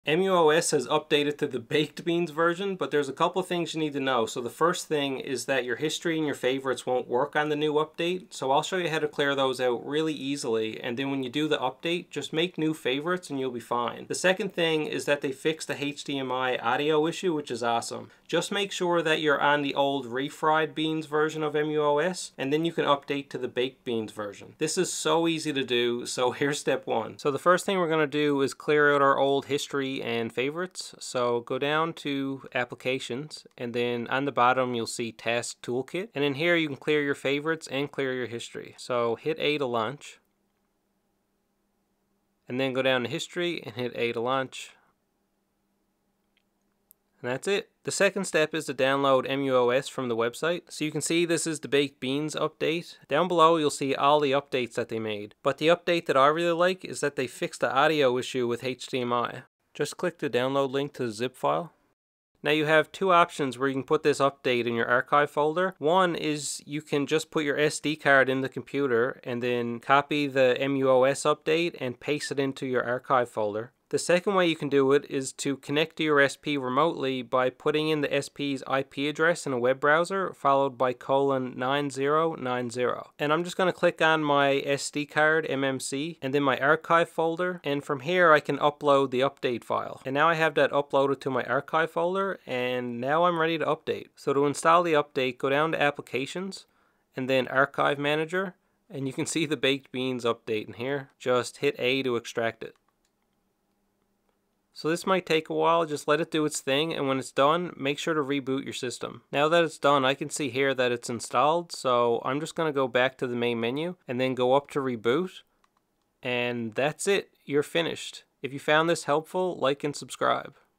muOS BAKED BEANS Update Quick Guide!!
Easy and fast guide for installing the muOS update to BAKED BEANS!!

Timecodes
0:00 NEED TO KNOW
0:27 Clear History & Favorites
0:47 muOS Baked Beans Download
1:02 SD Card Install Option
1:13 Connect Remotely Install Option
1:33 Install & Reboot